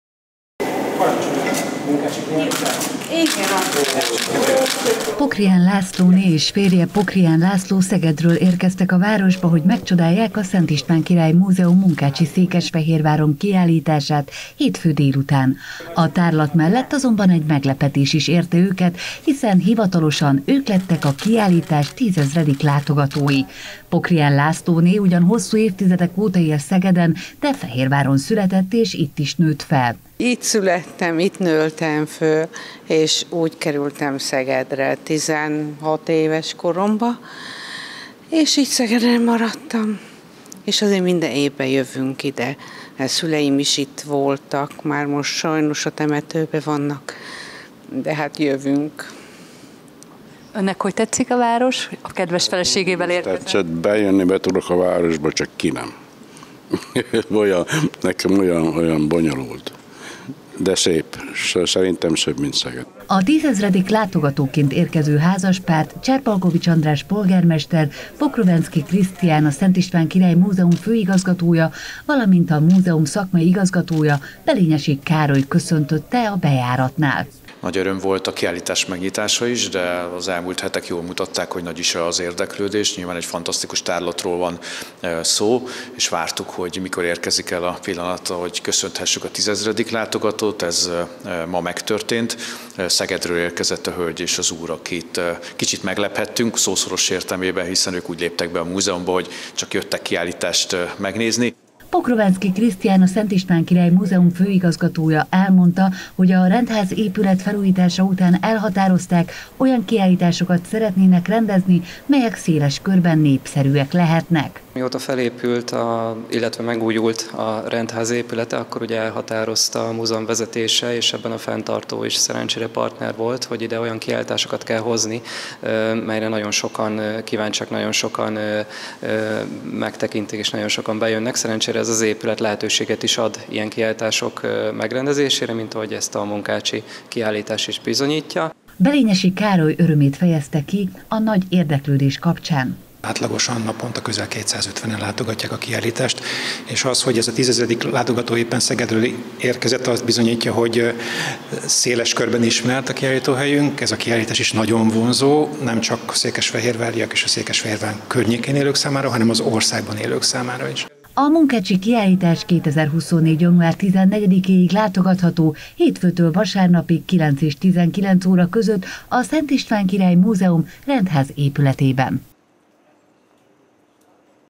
Tízezer látogató a Munkácsy kiállításon.
Szeptember közepén nyílt meg a Munkácsy Székesfehérváron – Válogatás Pákh Imre gyűjteményéből című kiállítás a Rendházban, melynek hétfőn délután köszöntötték a tízezredik látogatóit. A szerencsések Pokrián Lászlóné és férje, Pokrián László voltak, akik Szegedről érkeztek, hogy megnézzék a tárlatot.